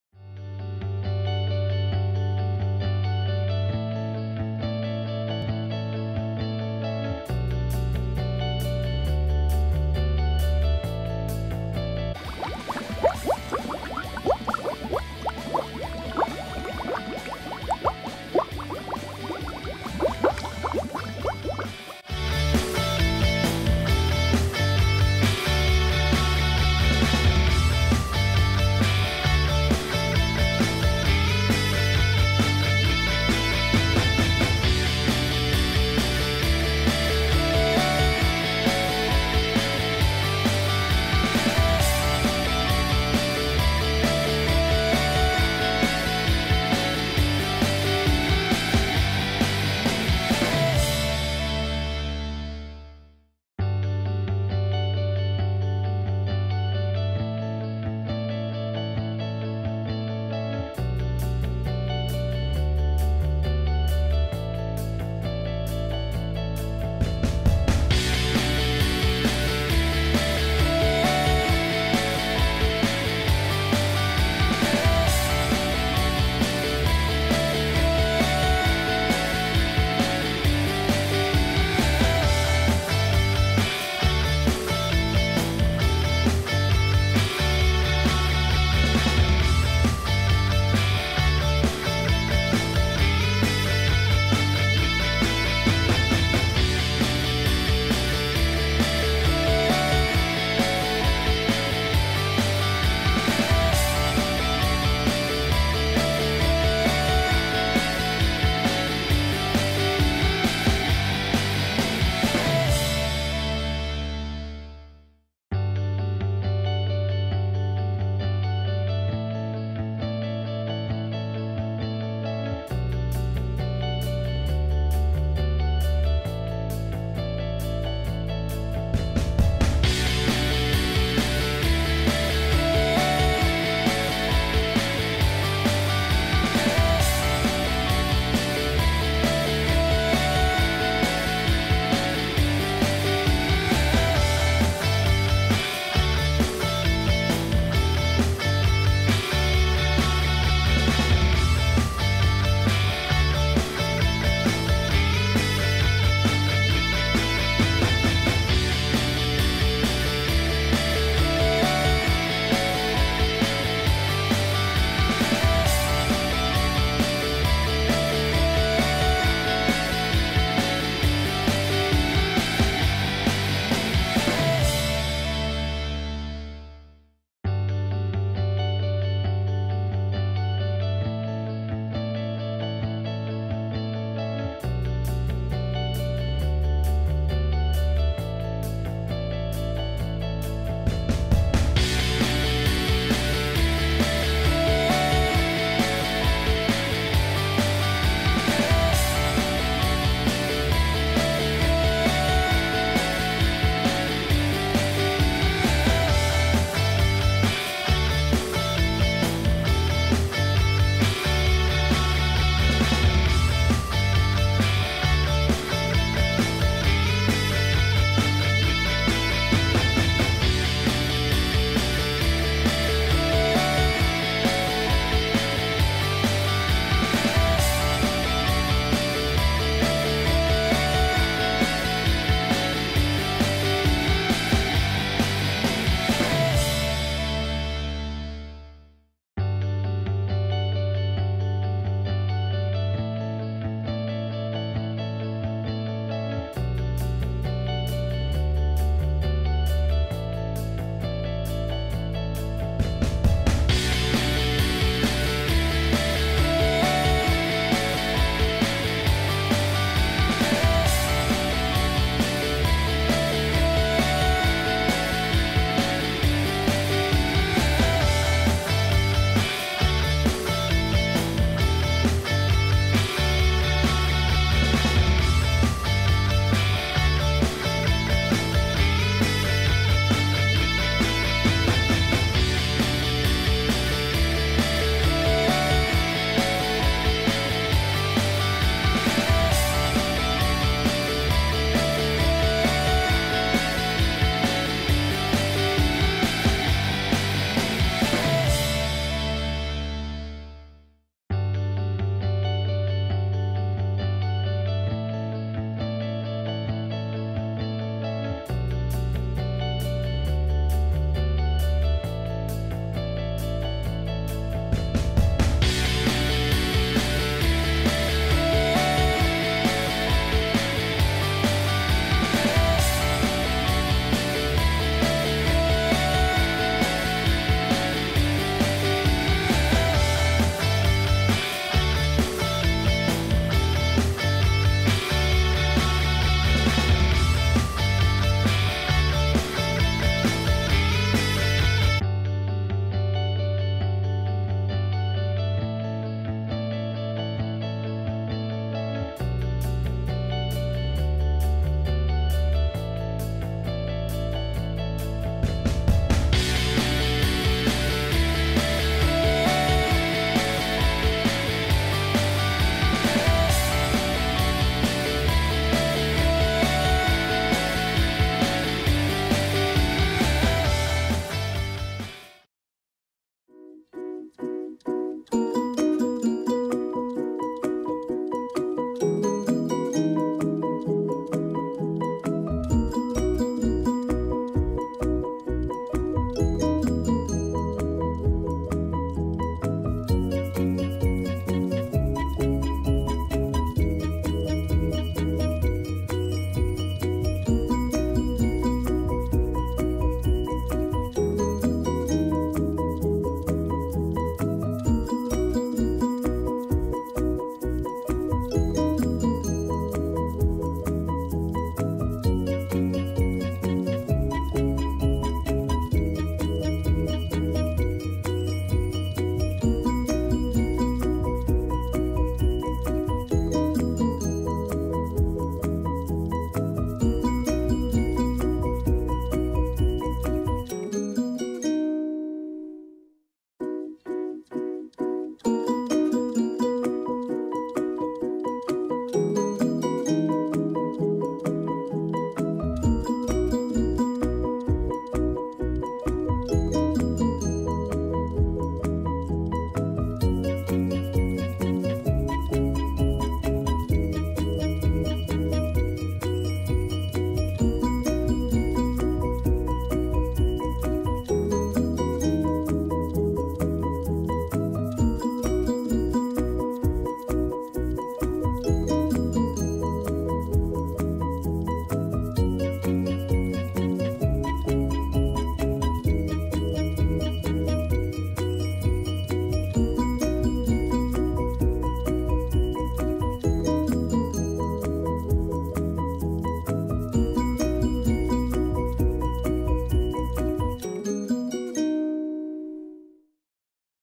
Let's review Japan!日本を見直そう！５熊本Kumamoto
日本を見直そう！の５回目をアップします。今回は熊本です。阿蘇の雄大な景色を中心にご紹介します。早く大震災の後遺症から脱することを祈りつつ美しい風景が戻ることを期待して紹介いたします。